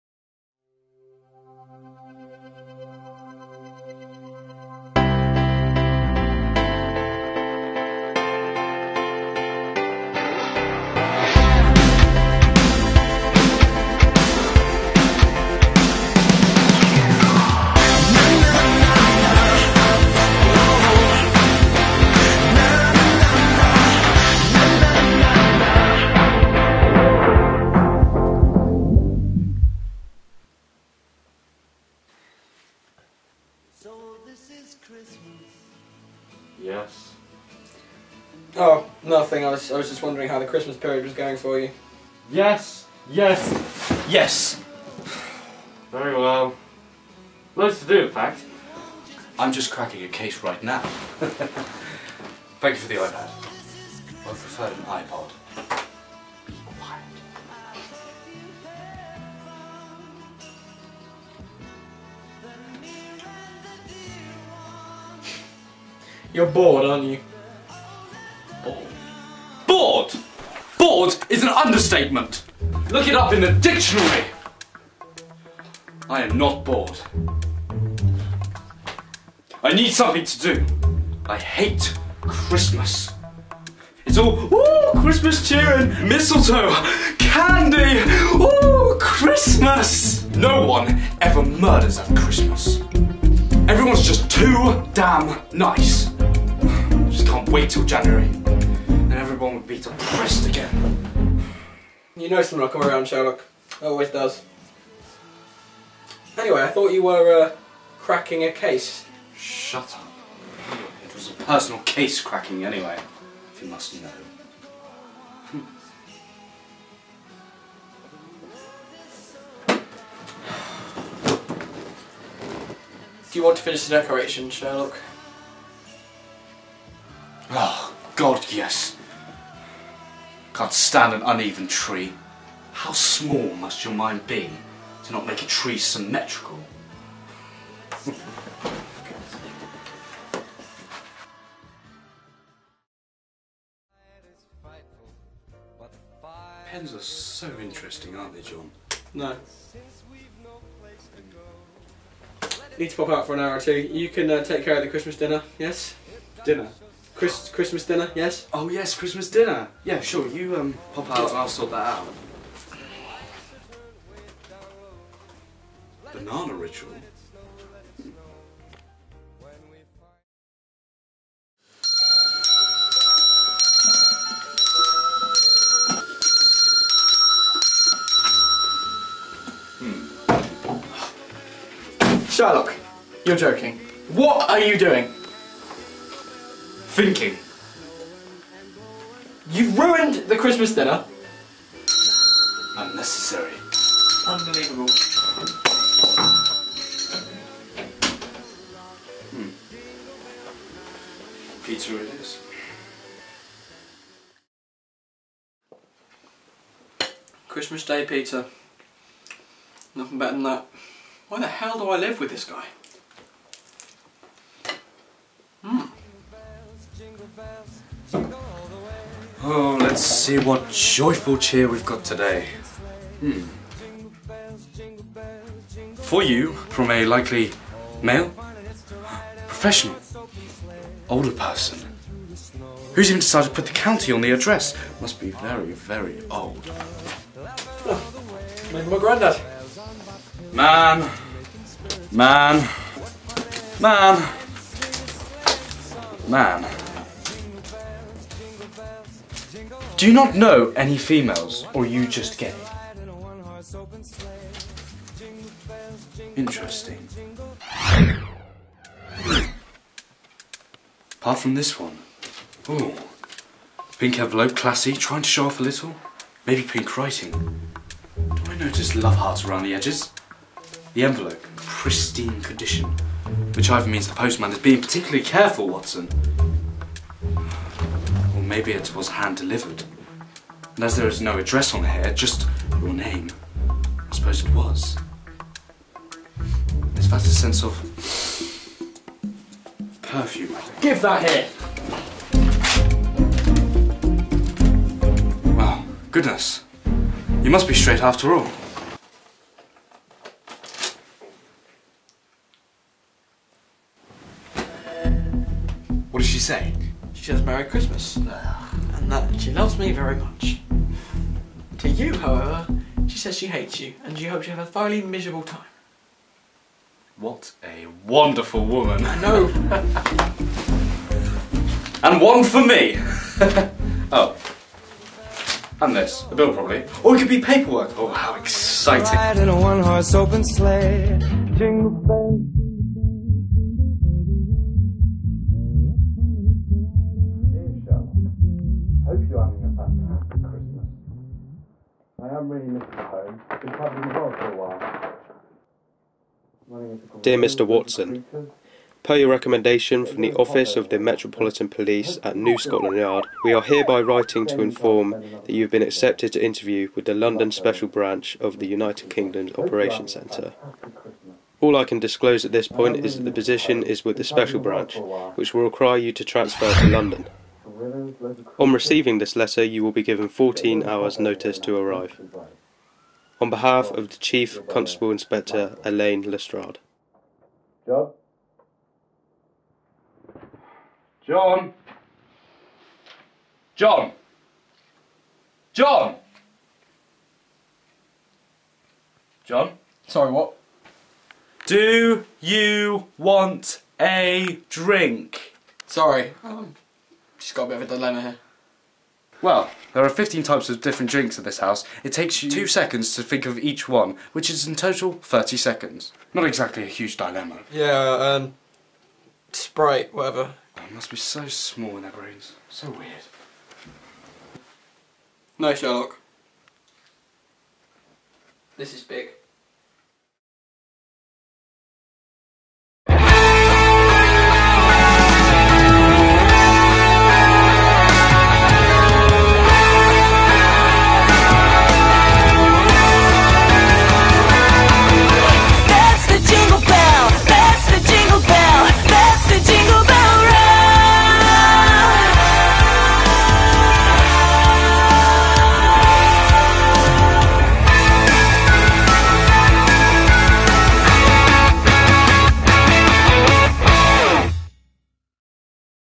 Sherlock - Christmas Special
Sherlock Returns for a short Christmas special, where it seems he isn't getting into the spirit of it...

2nd episode of the series, made with some of my mates, in our spare time. Nothing huge but I hope you enjoy it! Sherlock played by Joseph Marshall, Watson by Matt Higham. Written and Directed by Pete Wall.